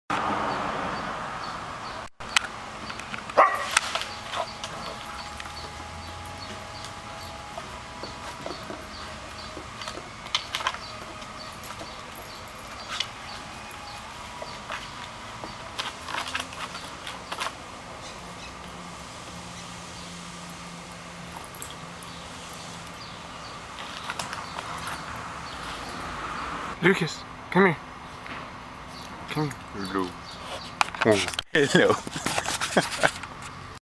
When Lucas Doesn't Get His Walk
Lucas doesn't appreciate early morning conference calls that mess with my real duties.